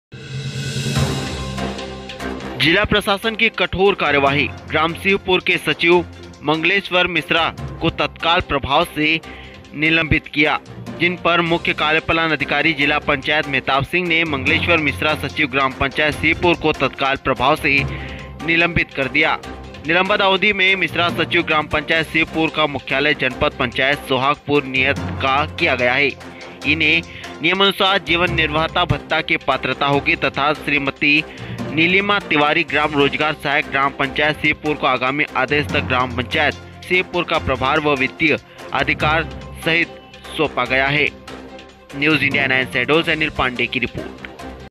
जिला प्रशासन की कठोर कार्यवाही सचिव को तत्काल प्रभाव से निलंबित
शहडोल 21 मई  मुख्य कार्यपालन अधिकारी जिला पंचायत शहडोल श्री मेहताब सिंह  ने ग्राम पंचायत सिंहपुर जनपद पंचायत सोहागपुर के सचिव श्री मंगलेश्वर मिश्रा द्वारा कोविड-19 संक्रमितो को क्वॉरेंटाइन सेंटर में शिफ्ट नहीं किए जाने, ग्रामीणों को वैक्सीन की जानकारी नहीं देने एवं उनका वैक्सीनेशन नहीं करवाने, जनता कर्फ्यू, कोरोना कर्फ्यू का पालन नहीं कराने, ग्रामों में कोरोना से बचाव का प्रचार प्रसार नहीं किए जाने, किल कोरोना अभियान में सर्वे दल का सहयोग नहीं करने जैसी गंभीर लापरवाही किए जाने तथा ग्राम पंचायत के अन्य विकास कार्यो में रूचि नहीं लेने के कारण सचिव श्री मंगलेश्वर मिश्रा का उक्त कृत्य मध्यप्रदेश पंचायत सेवा (आचरण) नियम 1998 का उल्लंघन होकर मध्यप्रदेश पंचायत सेवा (अनुशासन तथा अपील) नियम 1999 के तहत दंडनीय अपराध की श्रेणी में पाया गया है।

वीओ  1.1  जिस पर मुख्य कार्यपालन अधिकारी जिला पंचायत श्री मेहताब सिंह ने श्री मंगलेश्वर मिश्रा सचिव ग्राम पंचायत सिंहपुर को तत्काल प्रभाव से निलंबित किया है। निलंबन अवधि में श्री मिश्रा सचिव ग्राम पंचायत सिंहपुर का मुख्यालय जनपद पंचायत सोहागपुर नियत किया गया है एवं इन्हें नियमानुसार जीवन निर्वाह भत्ता की पात्रता होगी तथा श्रीमती नीलिमा तिवारी ग्राम रोजगार सहायक ग्राम पंचायत सिंहपुर को आगामी आदेश तक ग्राम पंचायत सिंहपुर का प्रभार व वित्तीय अधिकार सहित सौंपा  है।

 न्यूज़ इंडिया ए9 जिला रिपोर्टर शहडोल से अनिल पाण्डेय की रिपोर्ट